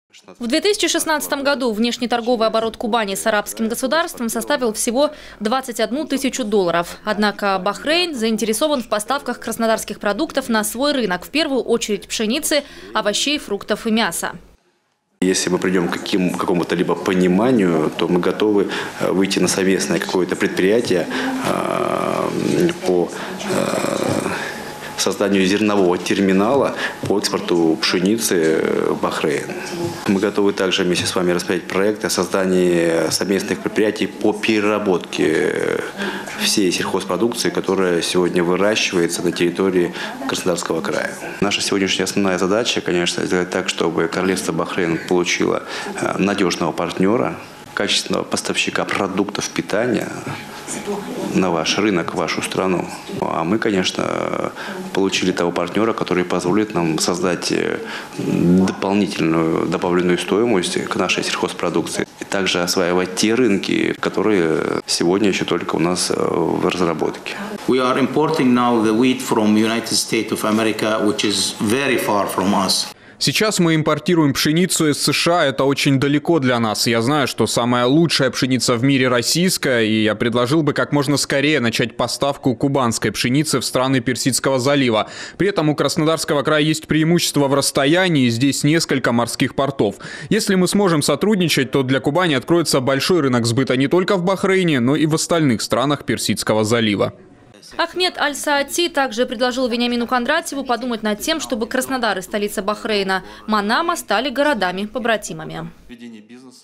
Краснодарский край готов поставлять пшеницу в Бахрейн. Новости Сочи Эфкате
О перспективах в сотрудничестве сегодня на встрече говорили губернатор региона Вениамин Кондратьев и посол Королевства в России Ахмед Аль-Саати.
Сочи. Новости.
Телеканал Эфкате.